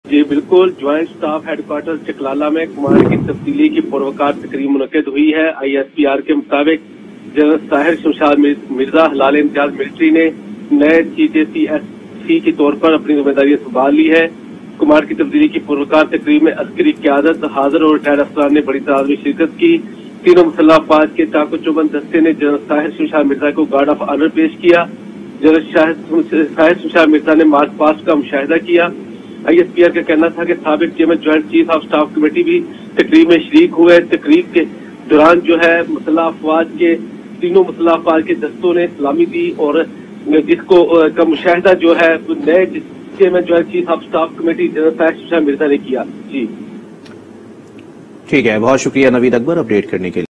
Command ceremony at the Joint Staff Headquarters | Lt Gen Sahir Shamshad took charge | Aaj News
Aaj TV Official:
shorturl.at/iBDNX